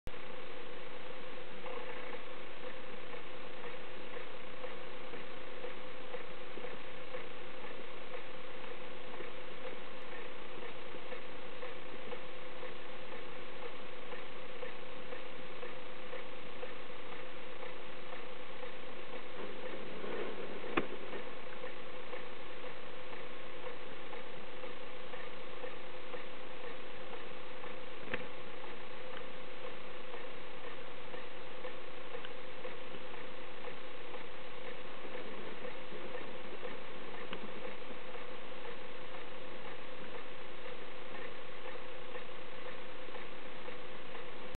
Lily 2
lily playing w/ her dryer sheet